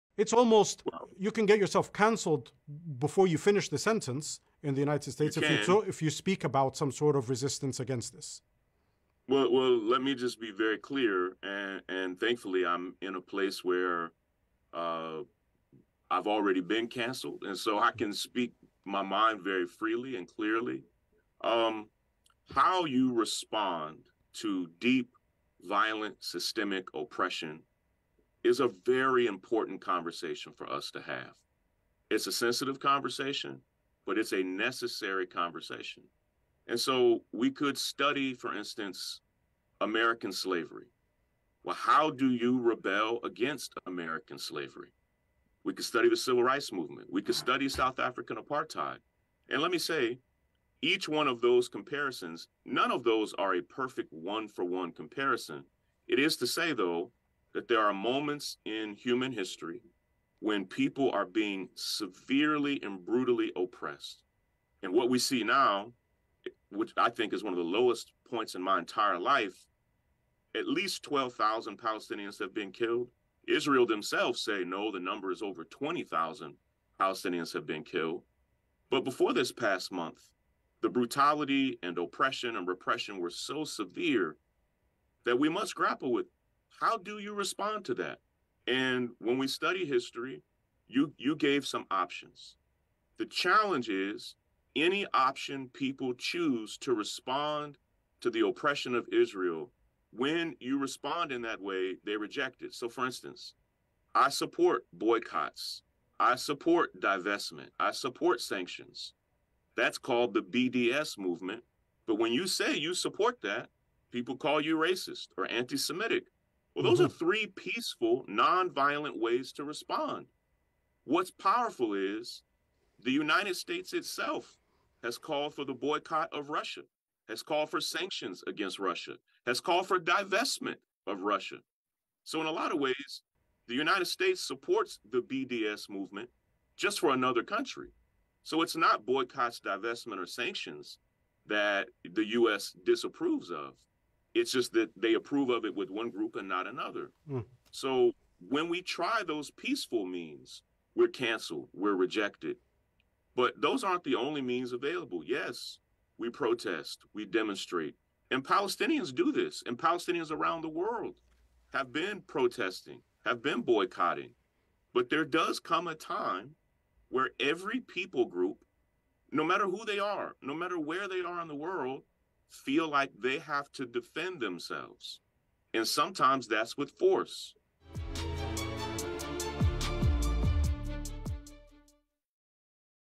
Shaun King speaking on Al Jazeera about what it means for oppressed people to fight back
This past November I was a guest on Al Jazeera and spoke to my friend and brother Jamal Elshayyal about the rights oppressed people have to defend themselves and fight back against their oppressors.